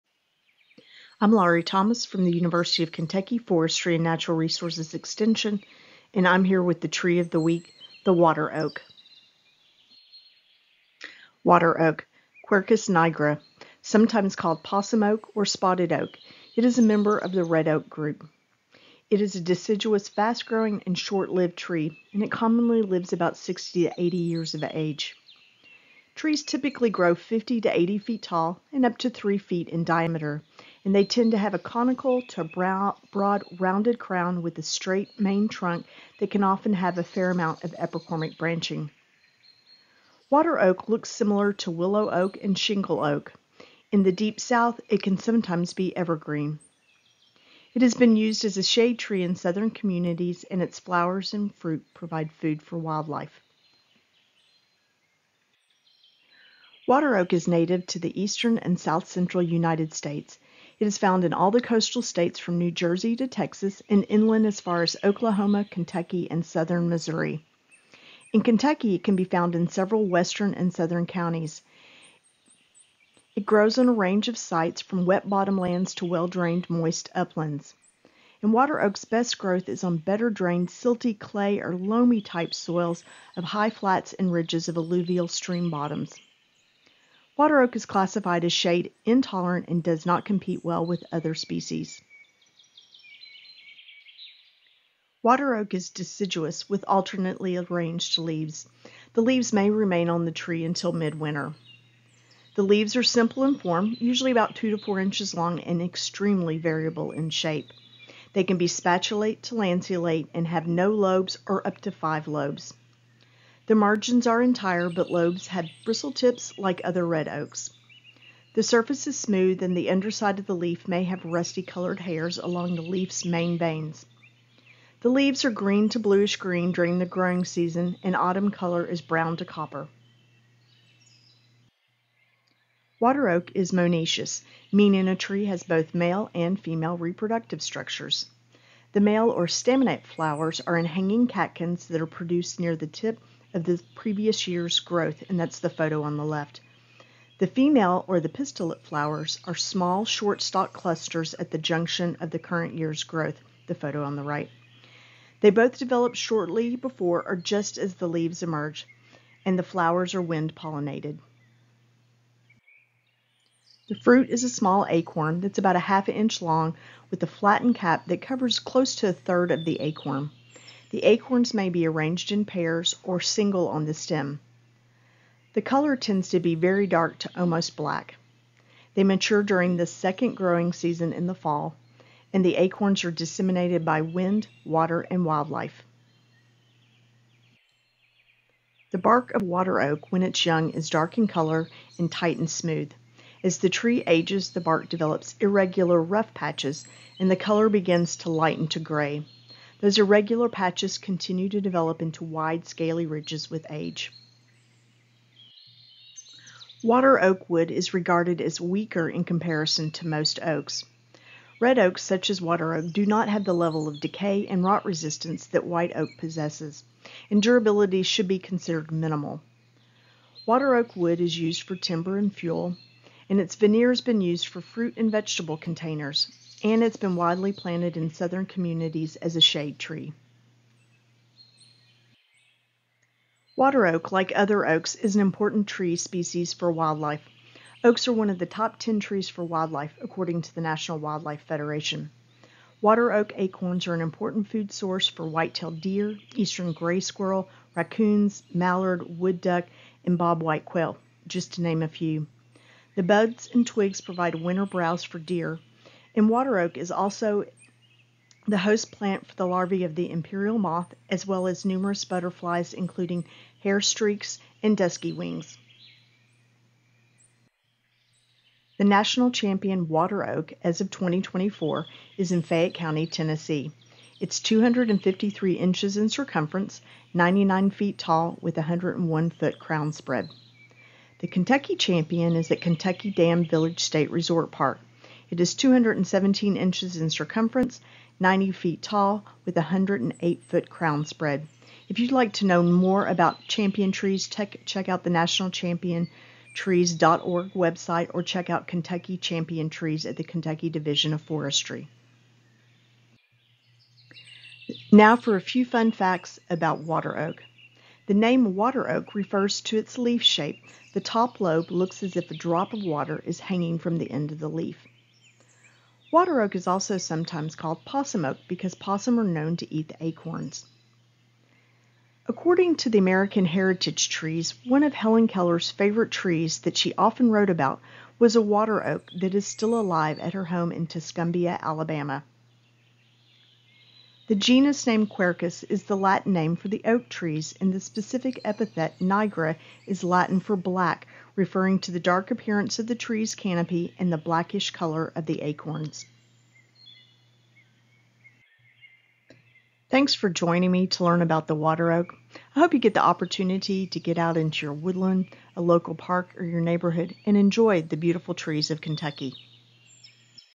Tree of the Week: Water Oak
Water oak (Quercus nigra) is used as a shade tree in southern communities that grows to 50-80 feet tall.  Its flowers and fruit provide food for wildlife.  It can also be called possum oak or spotted oak.